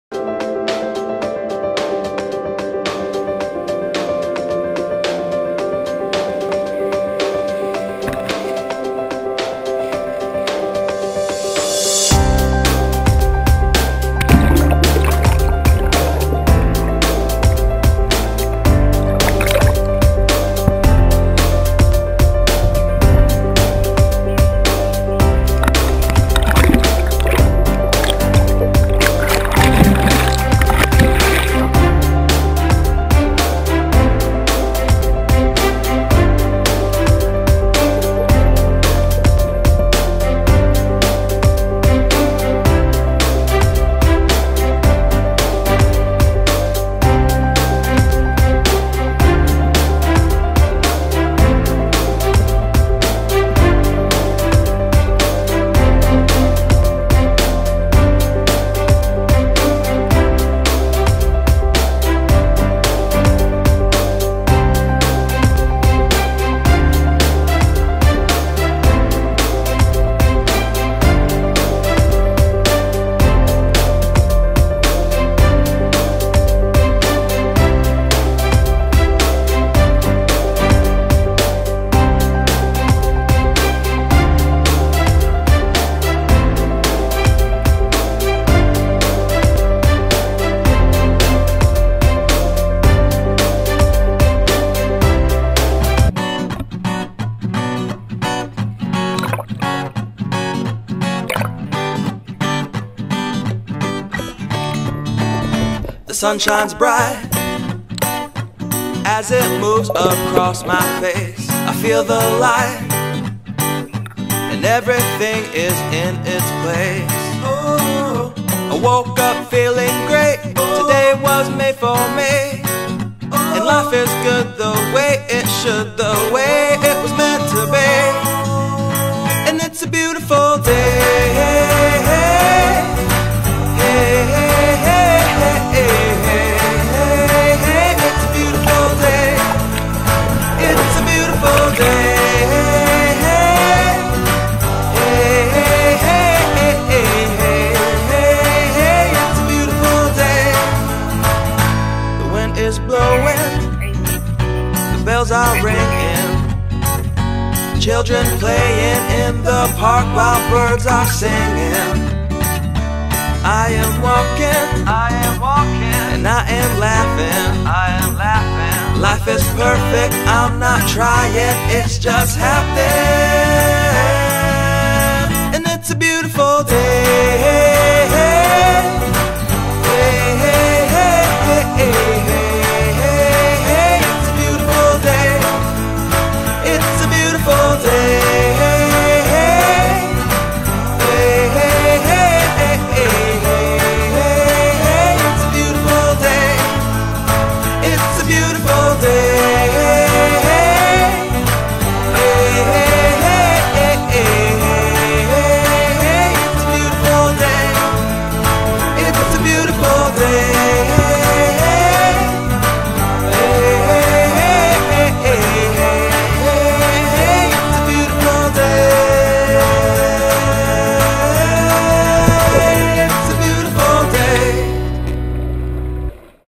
Seychelles 2013 - bluewaterfishing.eu
Walesharks, Mantas, Turtles and more ...
Author: Stephan Kreupl